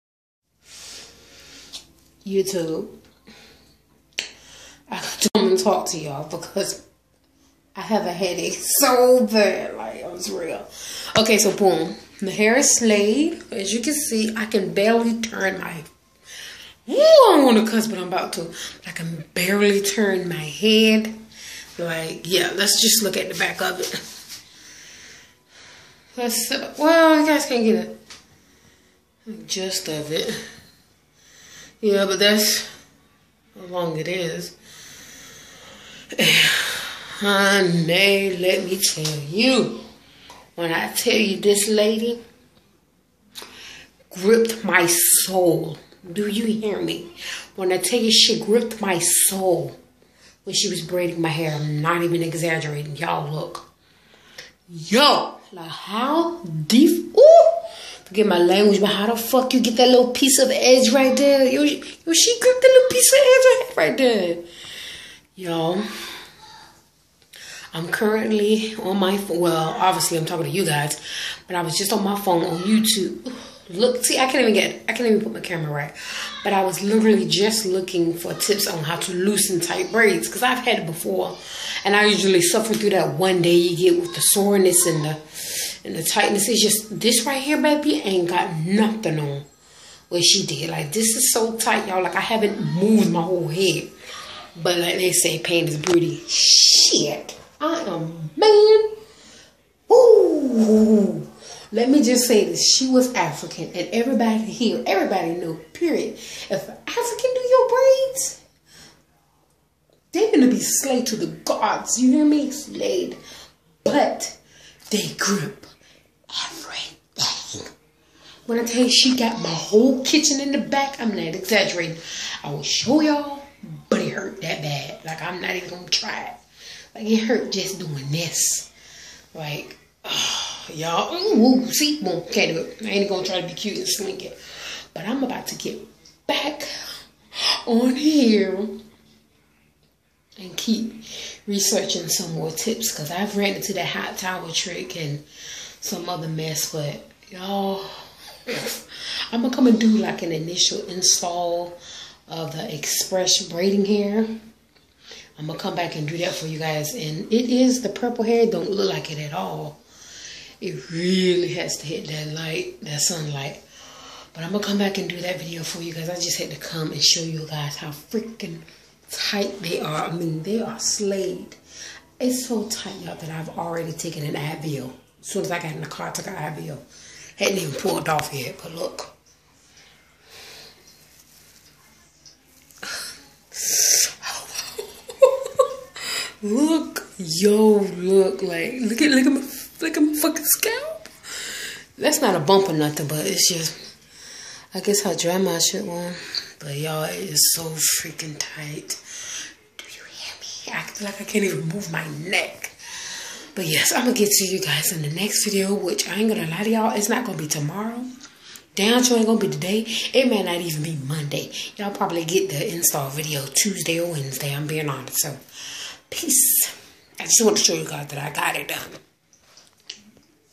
Braids By A African Lady~ Yoooooo My Head Is About To Blow
Heyyyyyyyy subbies&viewers thanx for watching. Just a small video on after I left the shop getting my hair braided. Omgggggg headache isn't the word you guys lol.